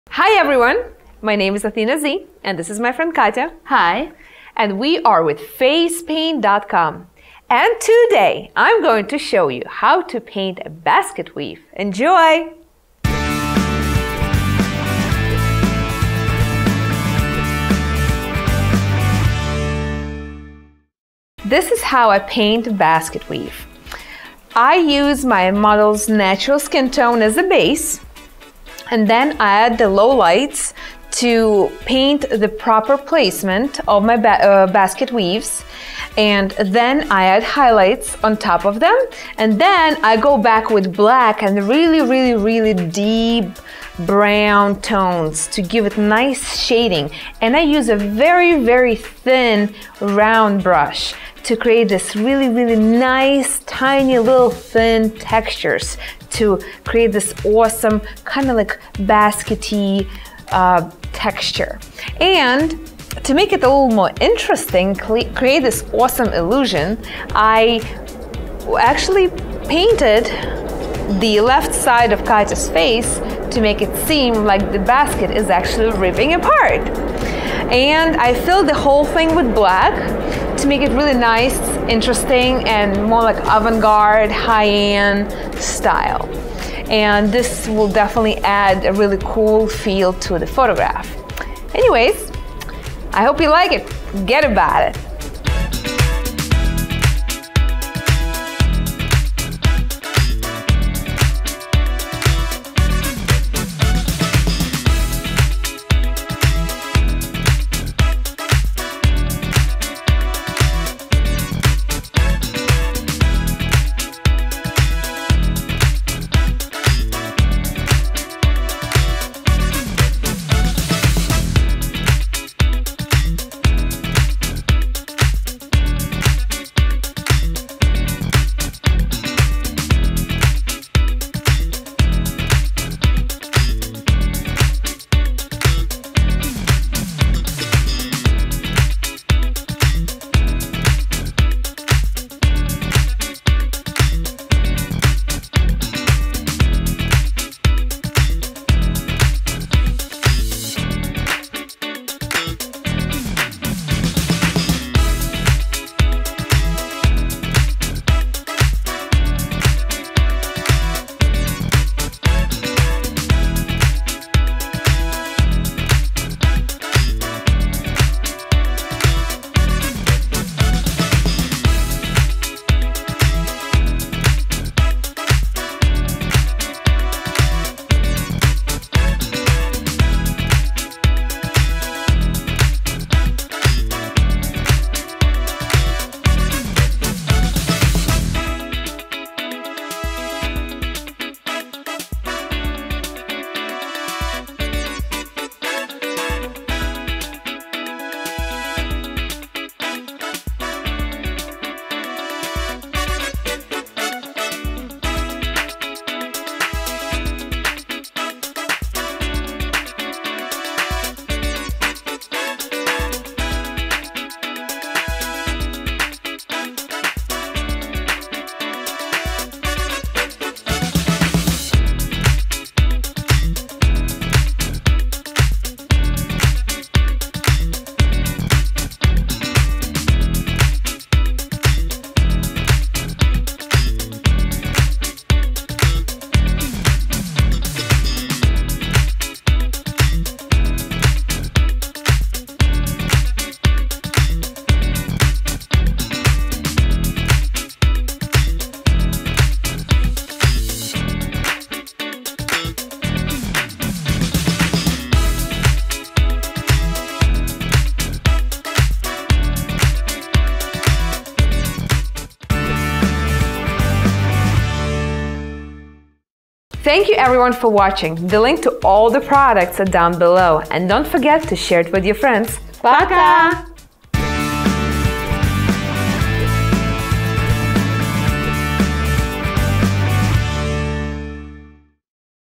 Basket Weave Design by Athena Zhe
Ever wanted to challenge yourself in making realistic designs using face paint? Athena Zhe has a new challenge for you today and it's amazing!

Check out Athena Zhe's Basket Weave design.

Products used :

Graftobian Paints

Tools